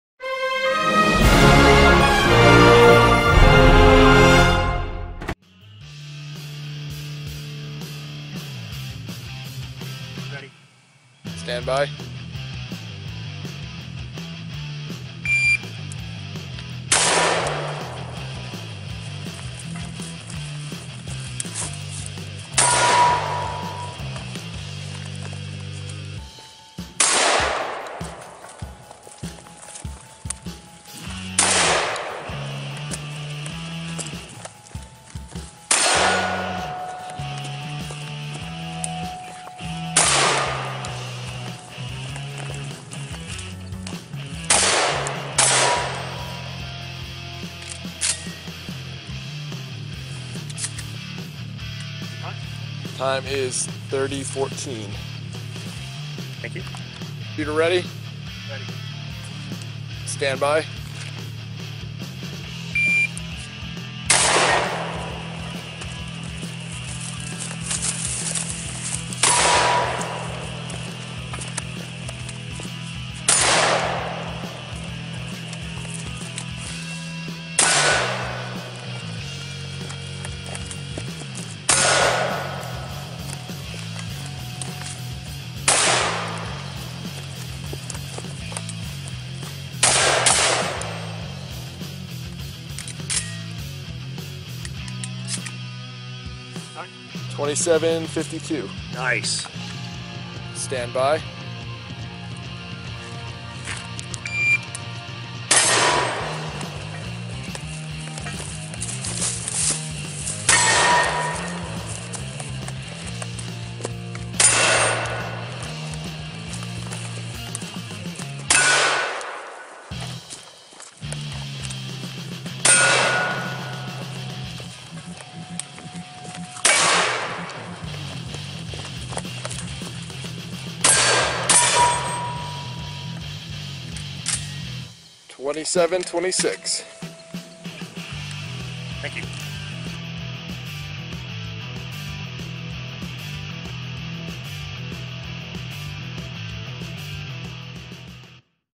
Shotgun Zig Zag Sept 2020
Just some moving and shooting with the 1301T.
Targets ~8y apart.